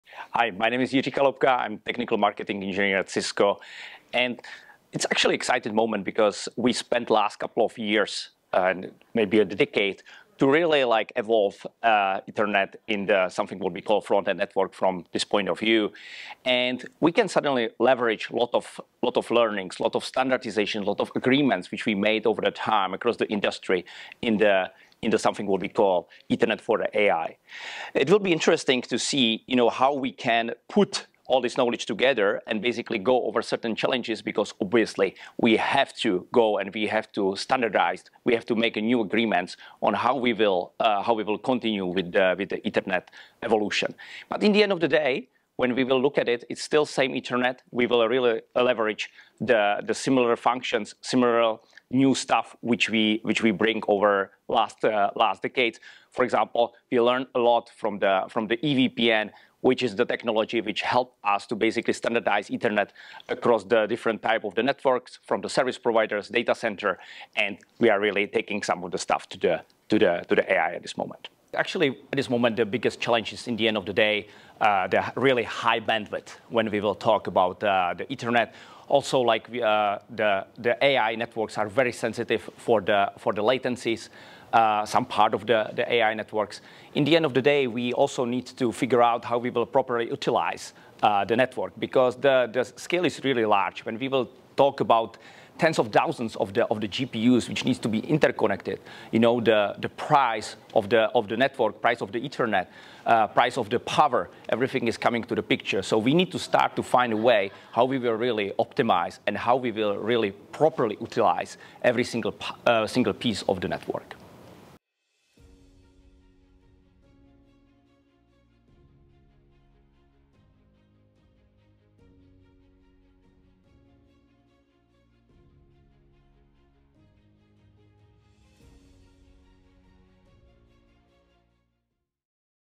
TEF24: AI-Driven Networks for the Future
to get executive perspectives from the show floor of the Ethernet Alliance's Technology Exploration Forum 2024: Ethernet in the Age of AI.

How is Ethernet evolving to meet the demands of AI networks?

Jiri Chaloupka, Principal Engineer Technical Marketing from Cisco Systems explains:

- Leveraging decades of knowledge and standardization from traditional networks to address AI networking challenges
- Adapting technologies like EVPN to standardize connectivity across different network types for AI applications
- Focusing on optimizing high bandwidth, low latency, and network utilization to support large-scale GPU interconnections